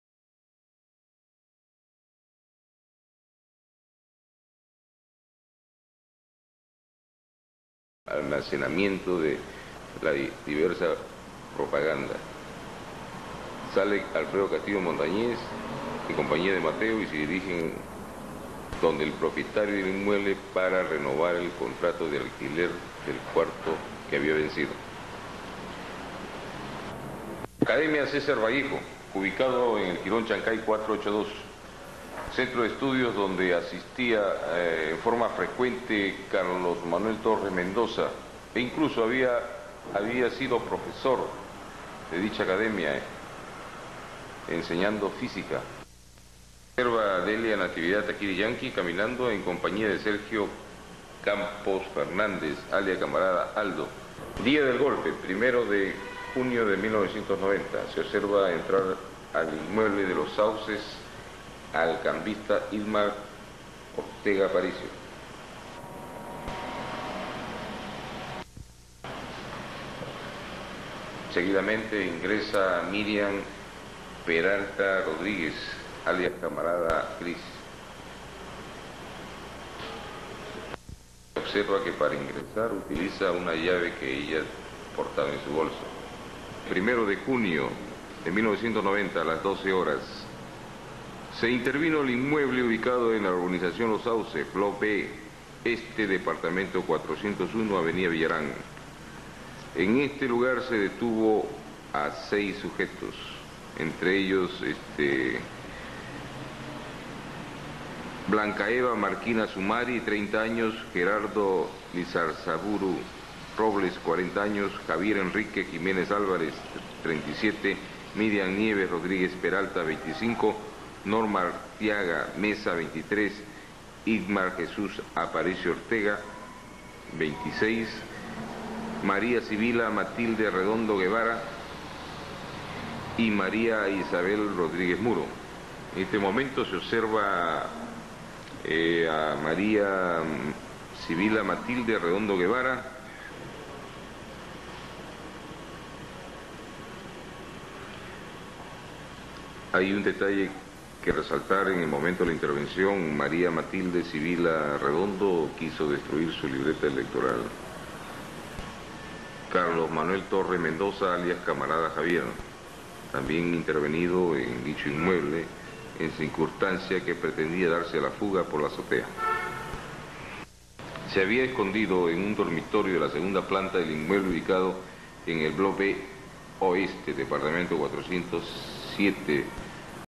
OPERACION GEIN ISA 3
Primera operación de inteligencia del GEIN (1990), empezó el 5 de marzo 90 y culminó el 1 de junio 1990. El director contra el terrorismo era el general PNP Edgar Suclla Flores.